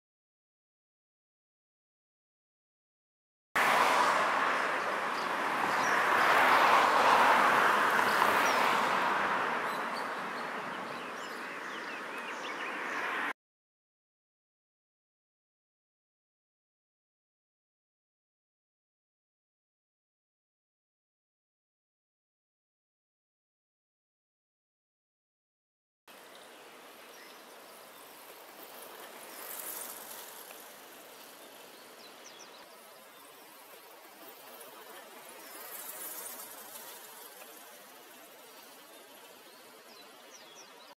カワセミのロング飛翔２態
イワツバメを撮っていて、カワセミが写っていました。飛翔距離、最初は、直線距離にして80m、次は25mです。